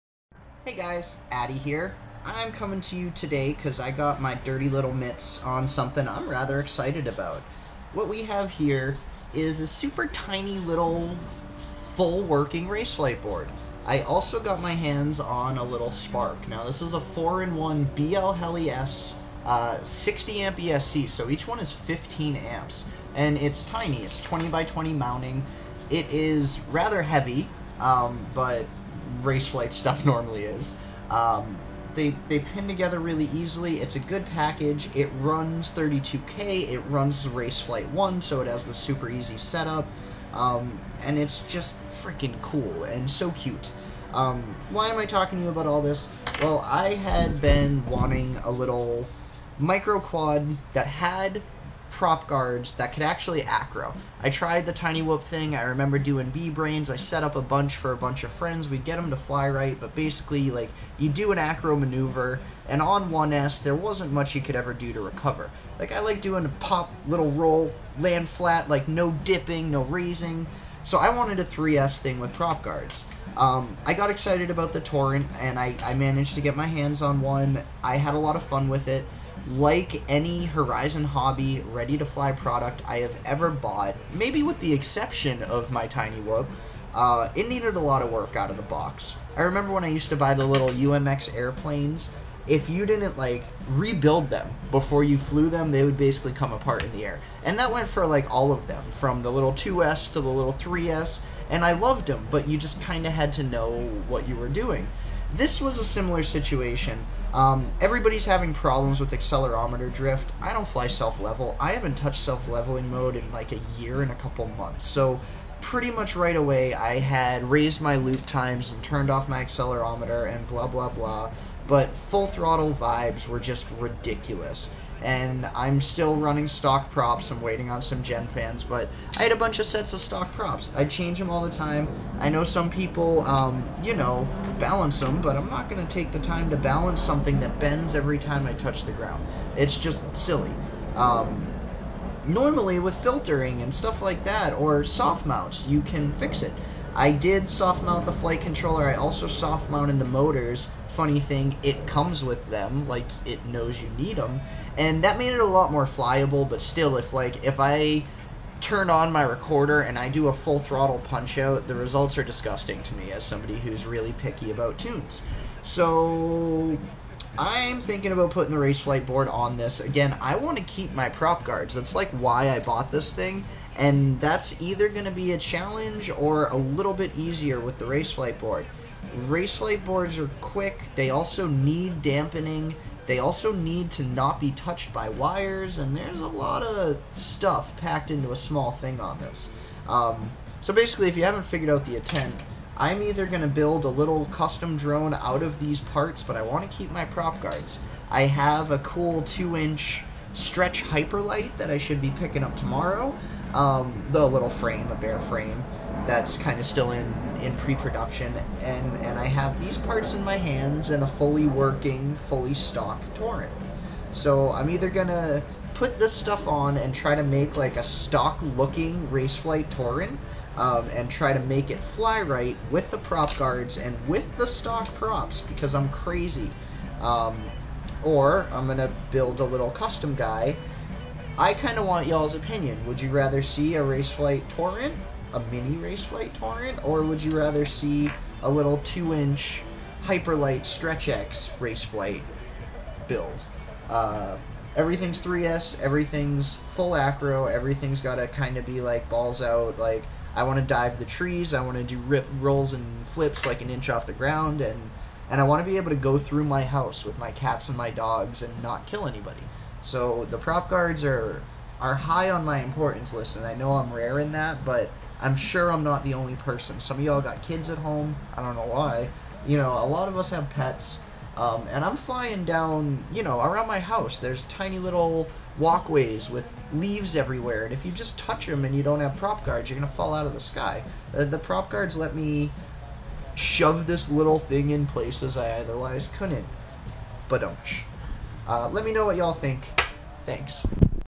RF1 Torrent- Spark and Millivolt???
Quick pole. Try to make a 2" stretched X out of torrent parts (with raceflight boards) or try to install raceflight board in stock looking torrent???